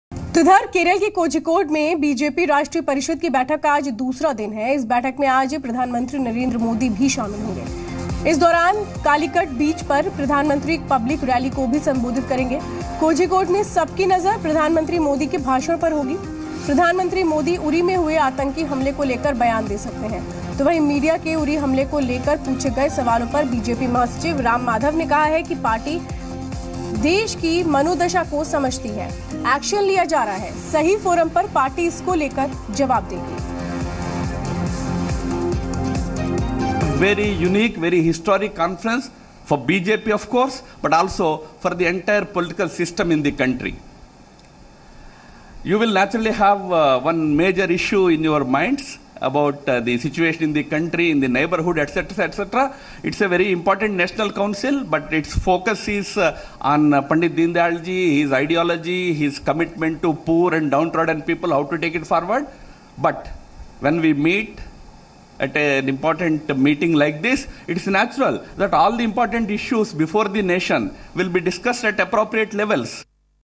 PM Modi will attend BJP meet and will also address public in Kozhikode, Kerala
PM Modi will attend BJP meet and will also address public in Kozhikode, Kerala. To know more watch the full news story